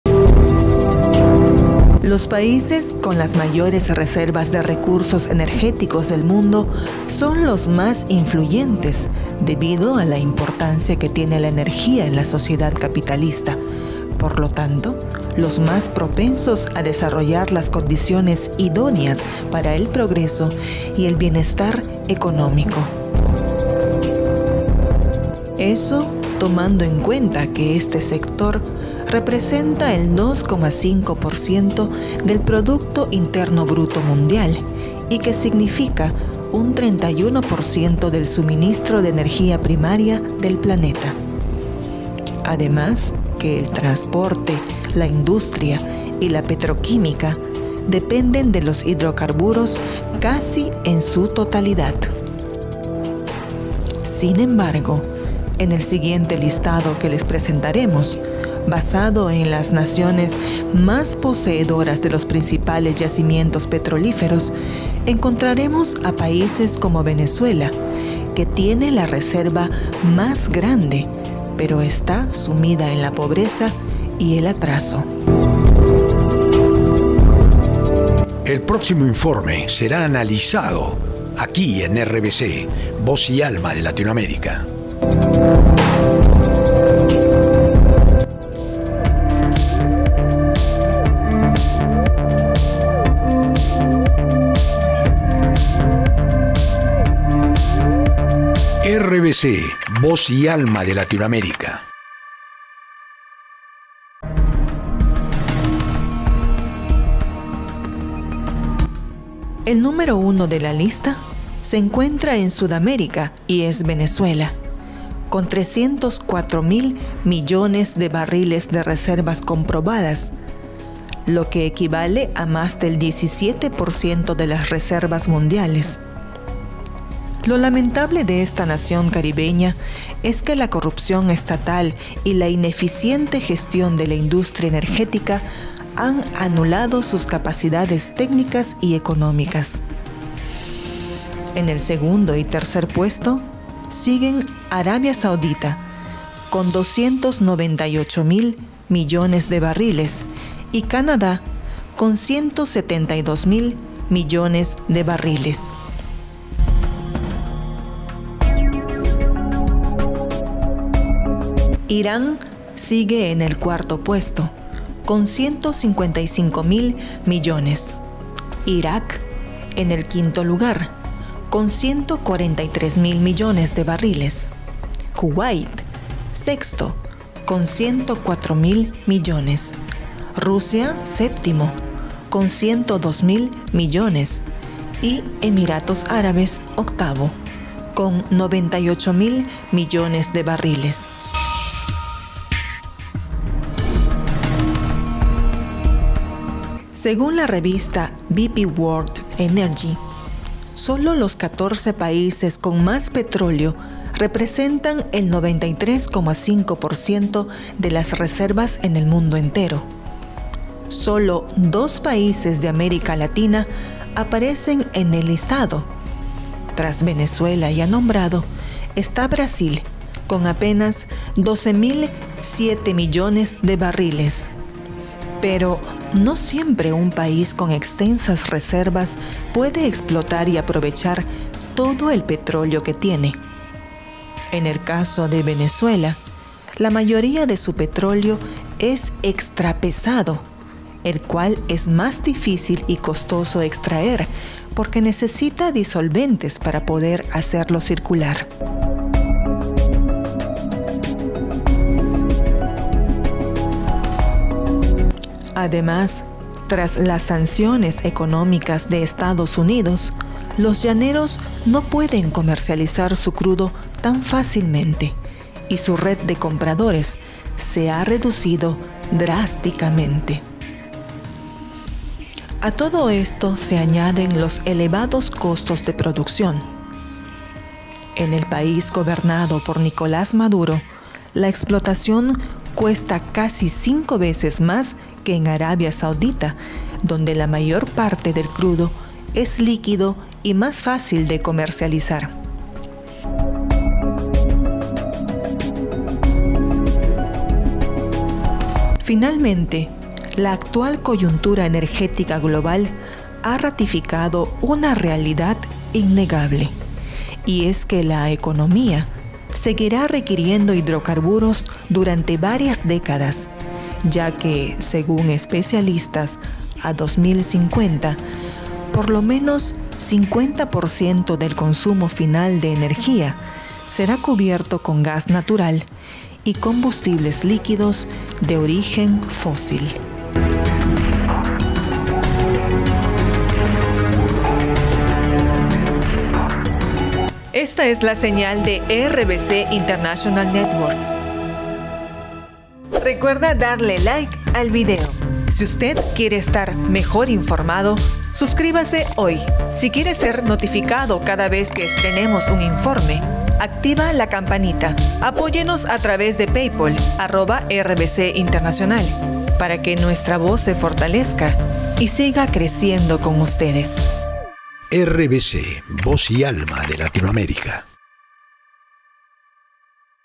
noticias Sepa usted cuáles son los países con mayores reservas de petróleo en el mundo.
Los países con las mayores reservas de recursos energéticos del mundo son los más influyentes debido a la importancia que tiene la energía en la sociedad capitalista, por lo tanto, los más propensos a desarrollar las condiciones idóneas para el progreso y el bienestar económico.
En el siguiente informe les presentaremos un listado de los países que más petróleo tienen en el mundo y el poder que ejercen en el mundo.

En este enlace a Amazon podrán adquirir:
"Pastillas para Levantar la Moral"
"LOS ONCE DÍAS DEL PERÙ"